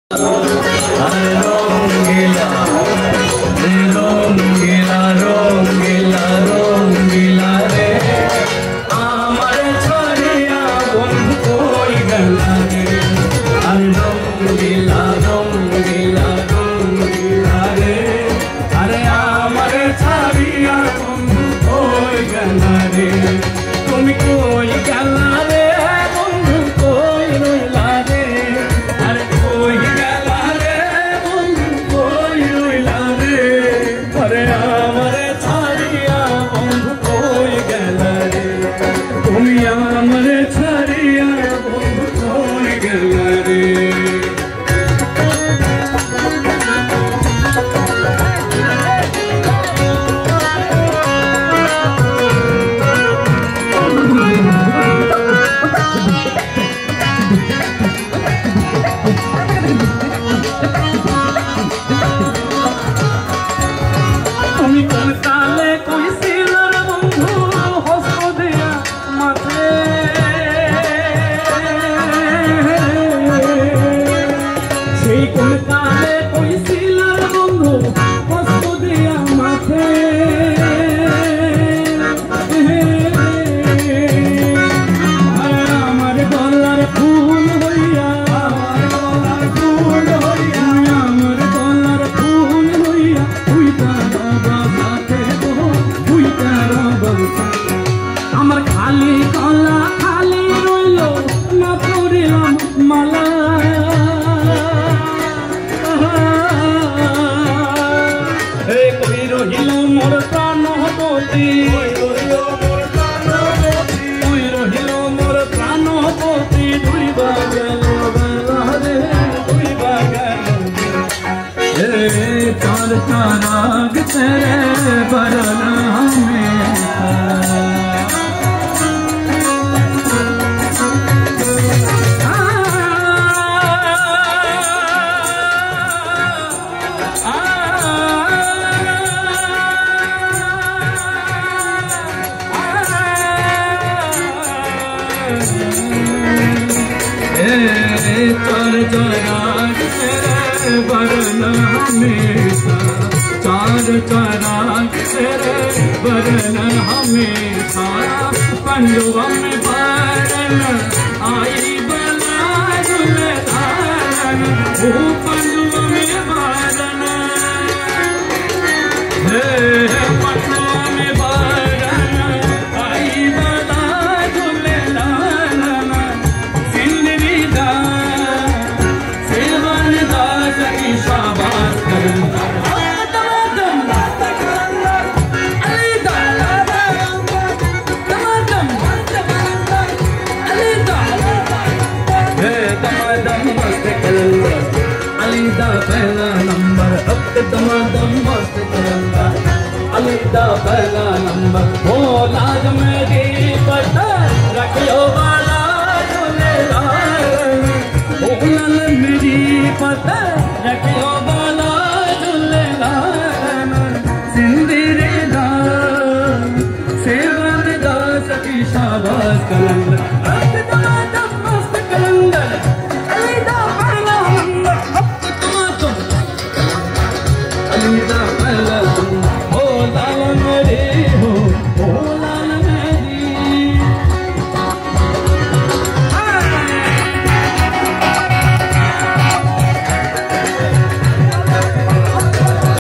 Comilla University Consert || SOROLA || Arnab Bhattacharya
Comilla University Consert
AIS Carnival 2023

On Stage- SOROLA
A folk Band.